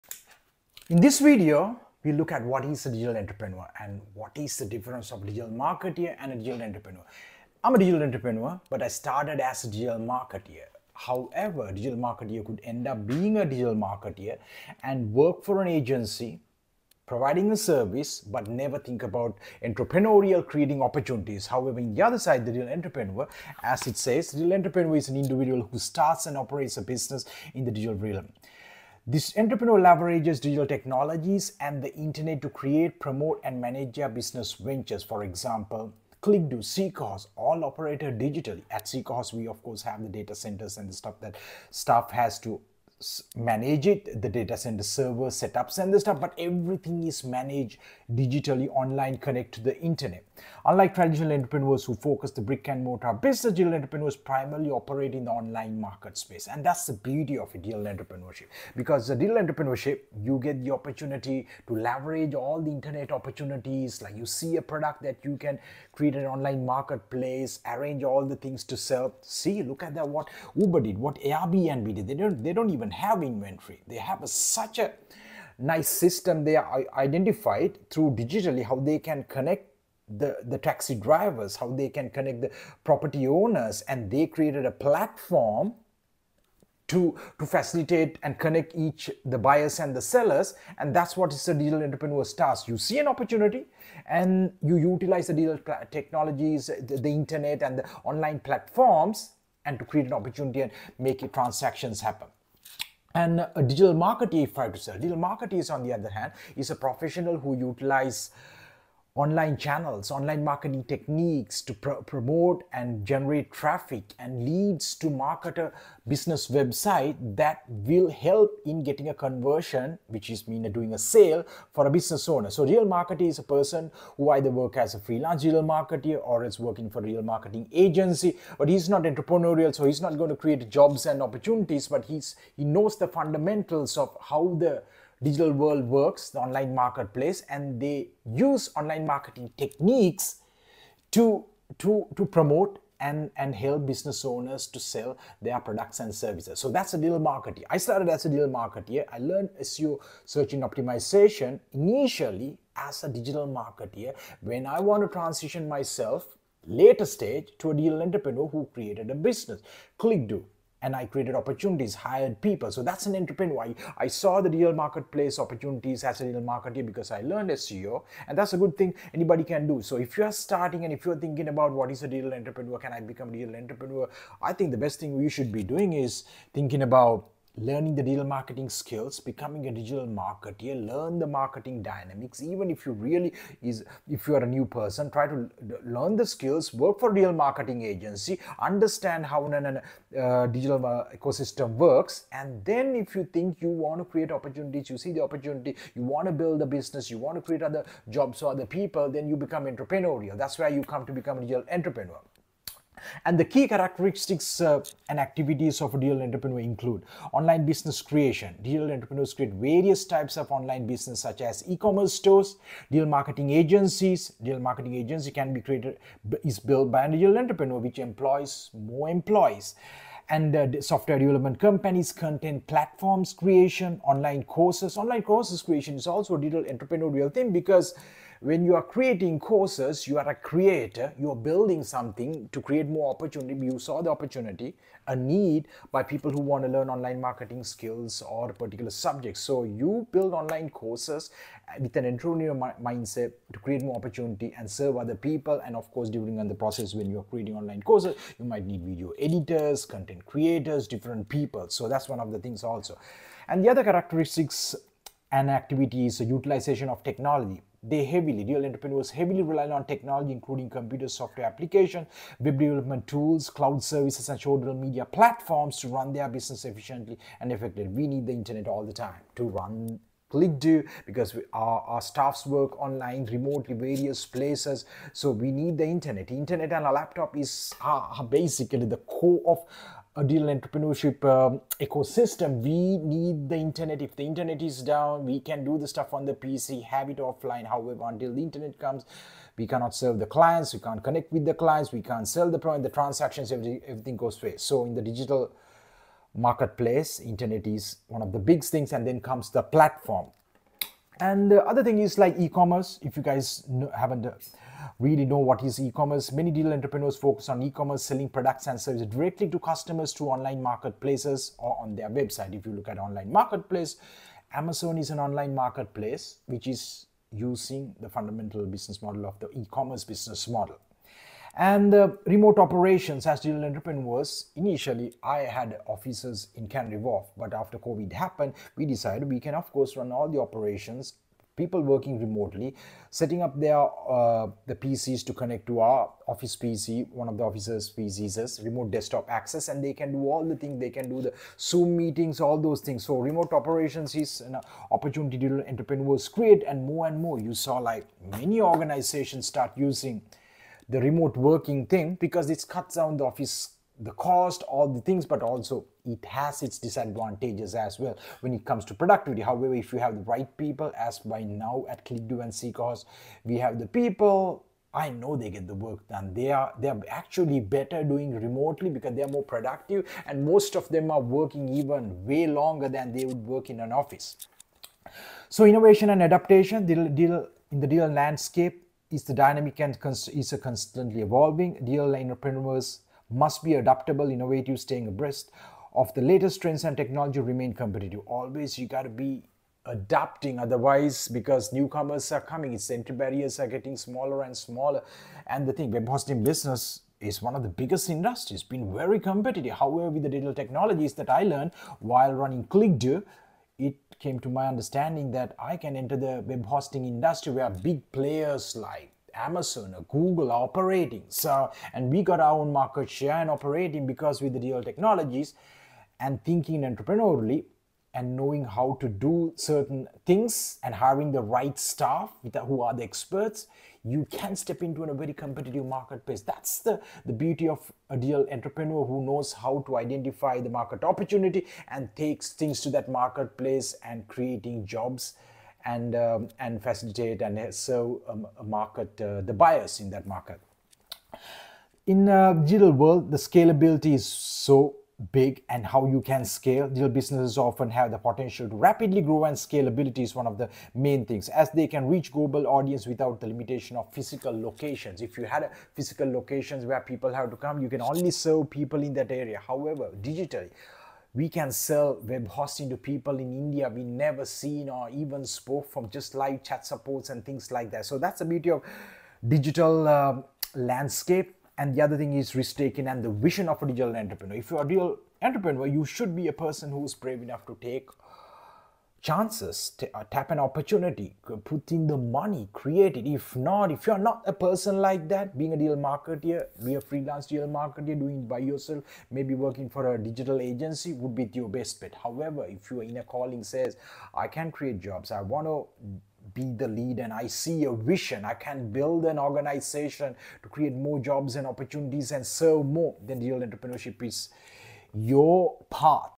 Who is a Digital Entrepreneur & What is the difference Vs. A Digital Marketer
In this video Fernando Raymond talks about who is a digital entrepreneur and how it’s different from a digital marketer.

Fernando started as a digital marketer and later become a digital entrepreneur. He found ClickDo digital agency in London and SeekaHost web hosting company in the UK.

Both are successful digital businesses and as a digital entrepreneur he saw the opportunity.

Who is a digital entrepreneur?

A digital entrepreneur is an individual who starts and operates a business in the digital realm. This entrepreneur leverages digital technologies and the internet to create, promote, and manage their business ventures. Unlike traditional entrepreneurs who focus on brick-and-mortar businesses, digital entrepreneurs primarily operate in the online space.

Key characteristics and activities of a digital entrepreneur include:
1. Online Business Creation: Digital entrepreneurs create various types of online businesses, such as e-commerce stores, digital marketing agencies, software development companies, content platforms, online courses, and more.
2. Utilization of Technology: They heavily rely on technology, including computers, software applications, web development tools, cloud services, and social media platforms to run their businesses efficiently and effectively.
3. Digital Marketing: Digital entrepreneurs use various digital marketing strategies to reach their target audience, build brand awareness, and promote their products or services. This may include social media marketing, search engine optimization (SEO), email marketing, content marketing, and online advertising.
4. E-commerce: Many digital entrepreneurs focus on e-commerce, selling products and services directly to customers through online marketplaces or their own websites.
5. Remote Operations: Digital entrepreneurs often run their businesses remotely, allowing them to work from anywhere with an internet connection and manage virtual teams located around the world.
6. Innovation and Adaptation: The digital landscape is dynamic and constantly evolving. Digital entrepreneurs must be adaptable and innovative, staying abreast of the latest trends and technologies to remain competitive.
7. Scalability: Digital businesses often have the potential for rapid growth and scalability, as they can reach a global audience without the limitations of physical locations.
8. Risk-taking and Vision: Like any entrepreneur, a digital entrepreneur needs to take calculated risks and have a clear vision for their business.

Examples of digital entrepreneurs include Jeff Bezos, the founder of Amazon, Mark Zuckerberg, the co-founder of Facebook, and Jack Dorsey, the co-founder of Twitter. However, digital entrepreneurship is not limited to building large-scale companies; it can also encompass small business owners and individual creators who leverage digital tools and platforms to generate income and impact online.